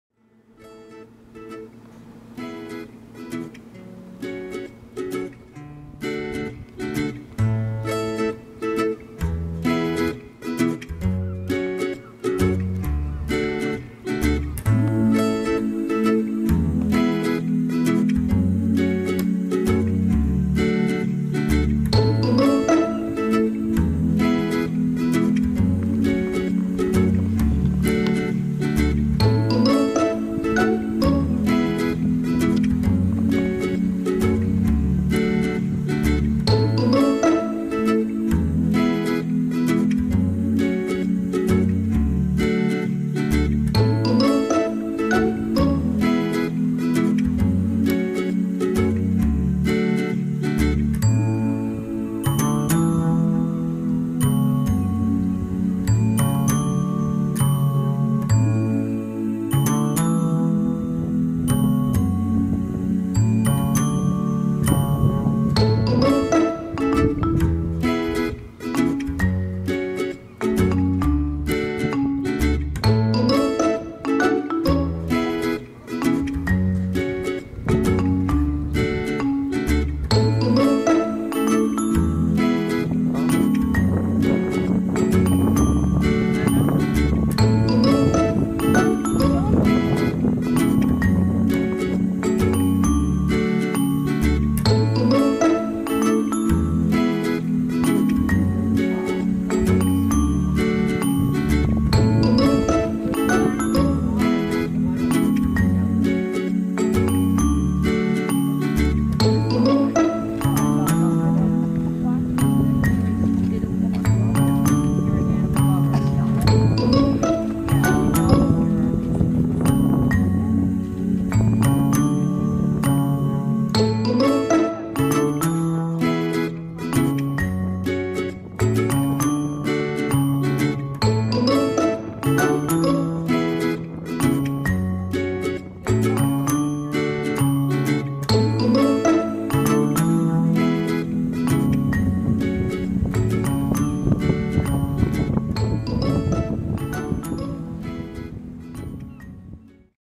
Seal at Quidi Vidi Wharf
A young hooded seal showed up on a sunny afternoon by the wharf of Quidi Vidi Village.  Lots more seals showing up likely from the influx of ice pans coming down further south and early this March in NL.  Love to see it relaxing and enjoy  warm basking in the sun in the east end of St. John's, Newfoundland, Canada.